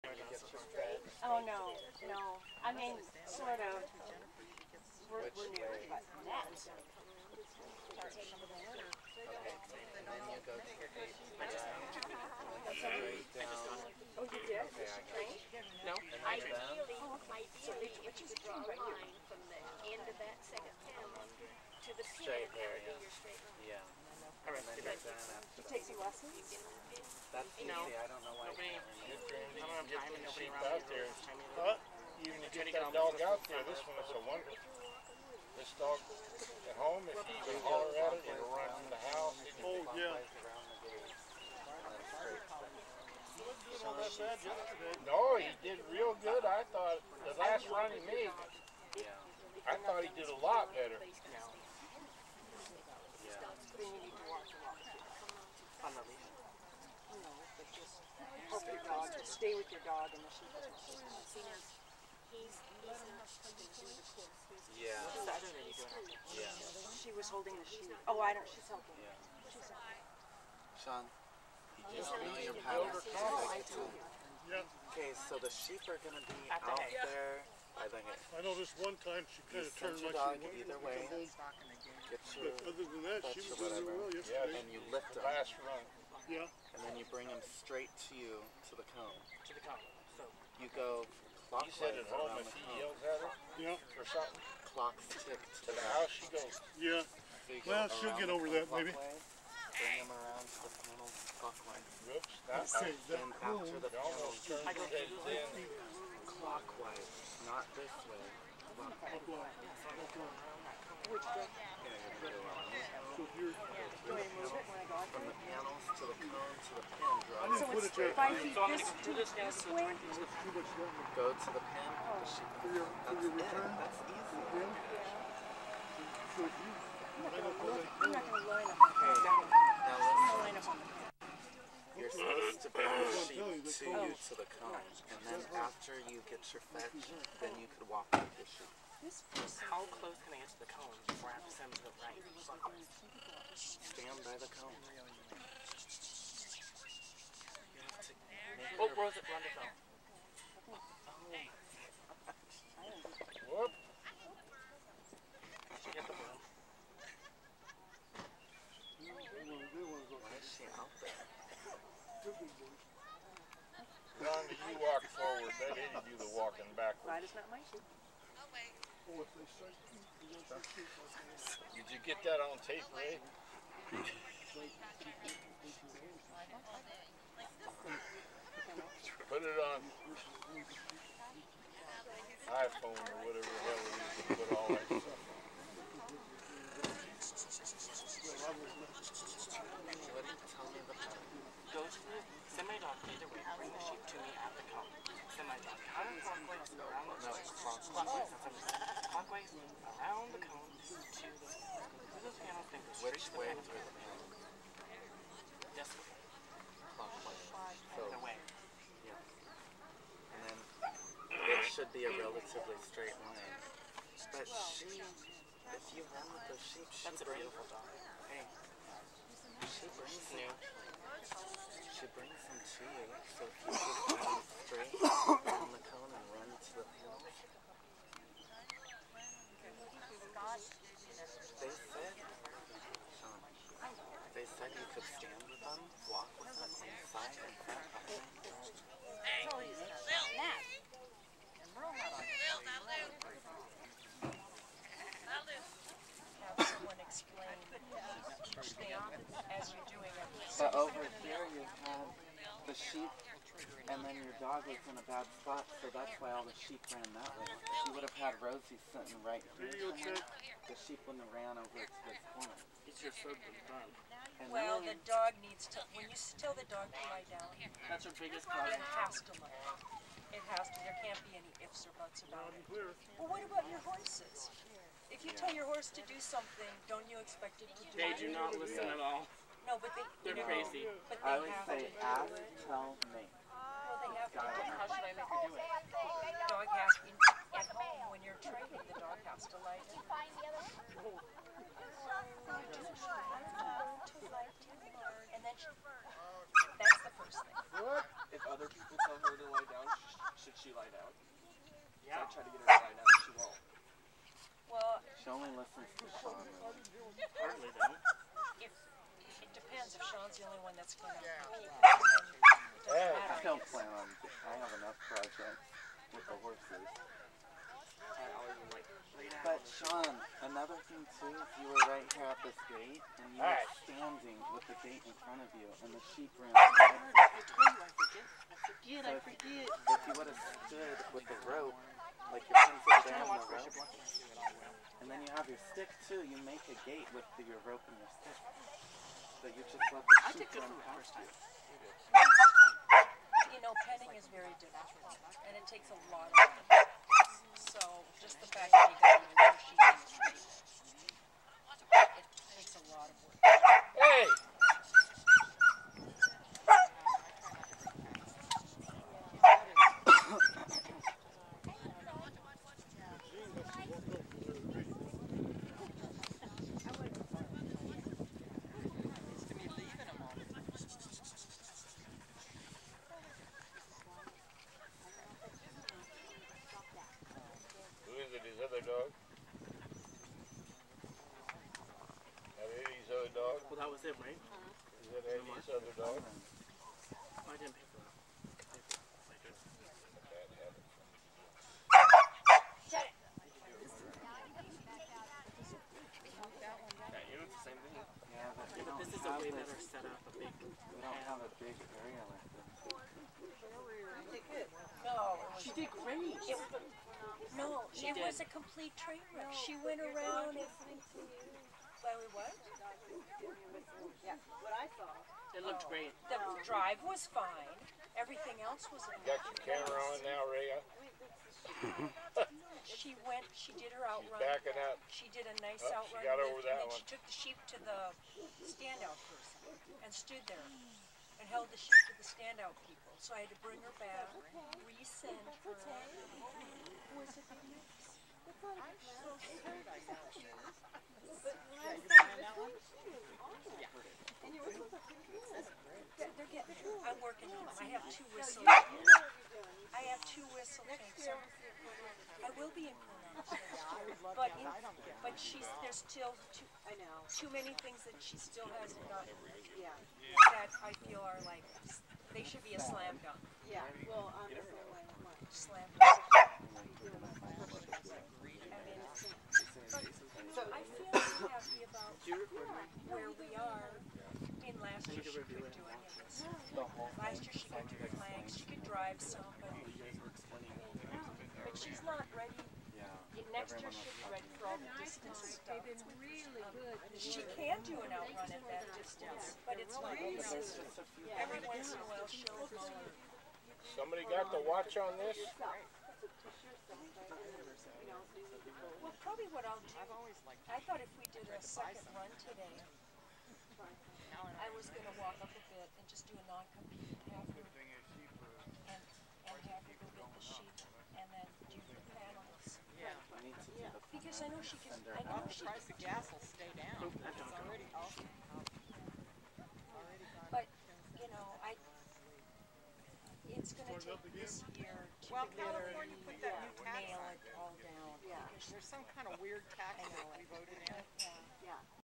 LILLY 2 YEAR OLD BORDER COLLIE, feb, 8th, 2015 Strathmore Ca
Lilly & Eddy Aldridge,   getting Lilly jazzed up in the field at the SJVBCA fun day today.     First time trying an out run with her,   she couldn't wait for me to get to my spot.
    I just wanted to get her jazzed up and working around set out people and dogs.        She handled things pretty well